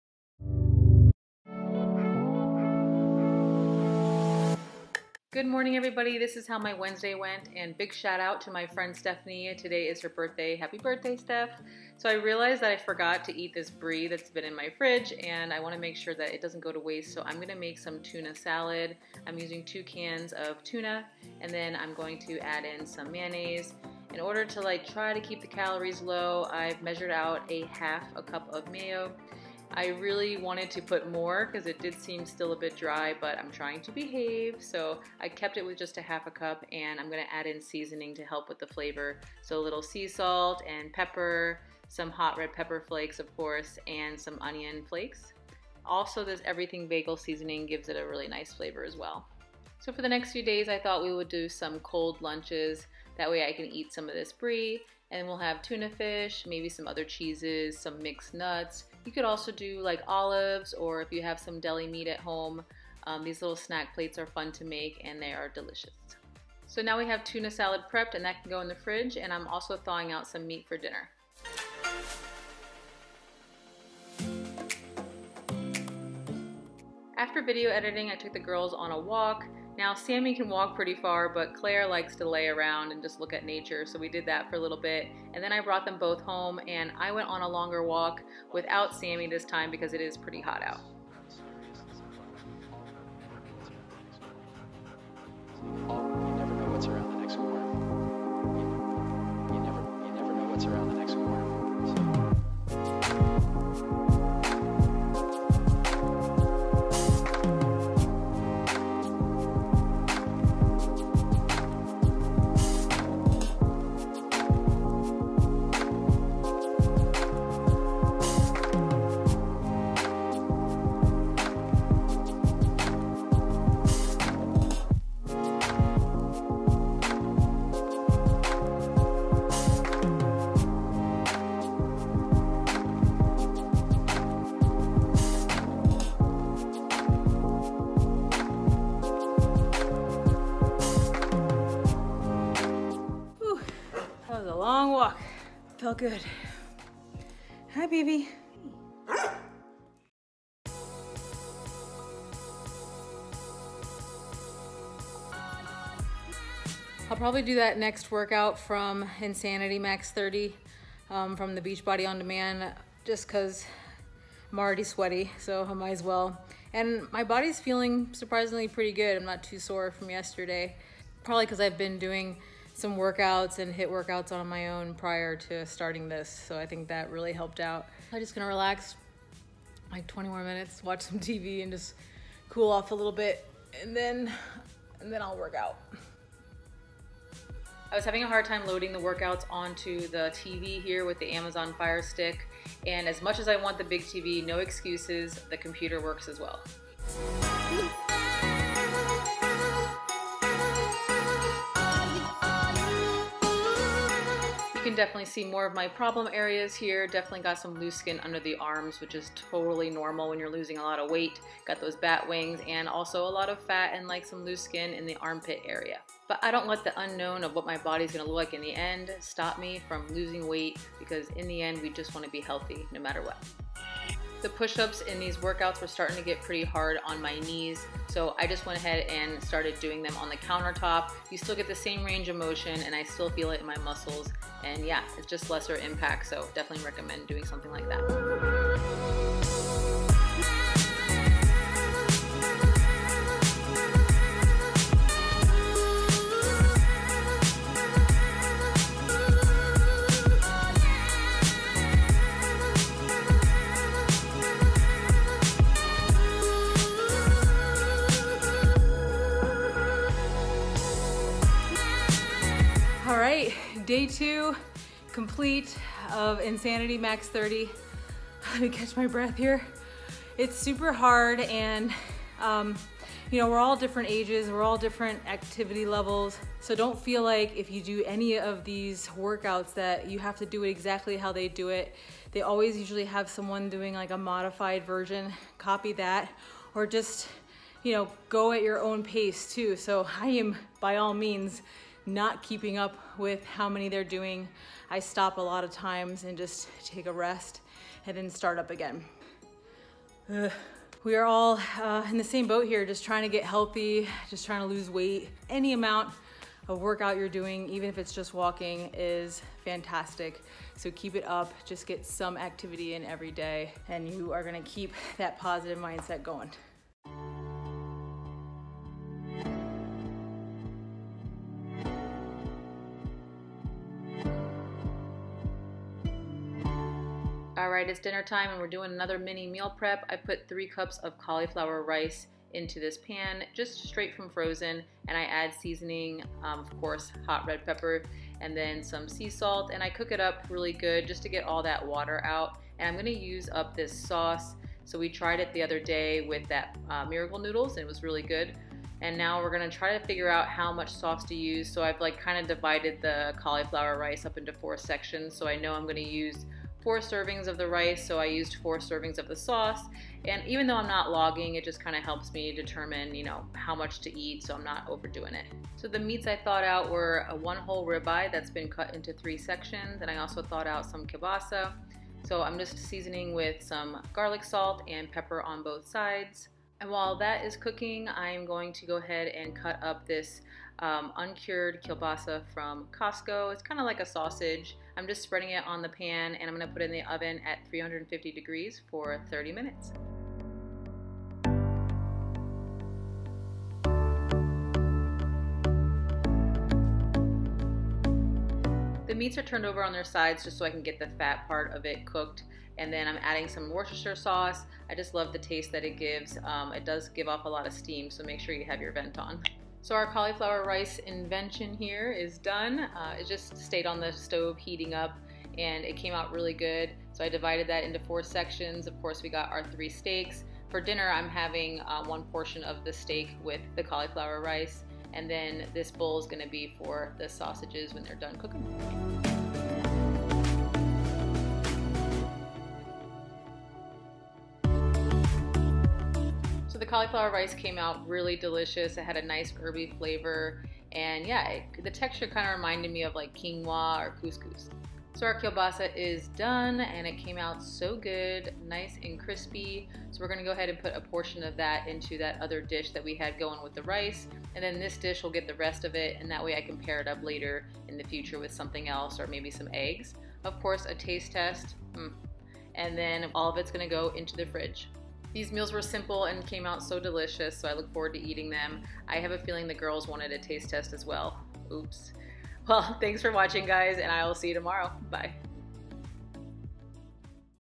Day 17 | Daily Workouts & Meal Preps | Low Carb Keto
Hey everyone! Hope you all are staying safe and sticking to your weight loss journeys as much as possible. I know these times are tough. I hope my daily videos are giving you tips and motivation to keep pushing. Meal preps and workouts are about all we can control right now. Hang in there! xo

Age: 35
Height: 5'6
Starting Weight (2018): 263 lbs
Lowest Weight (2019): 166-173 lbs

Project Comeback (again) :)
Start: 205 lbs
Week 1: 192 lbs
Week 2: 189 lbs

Weigh-in's on Mondays

[0:00]:
[2:36]:
[5:20]: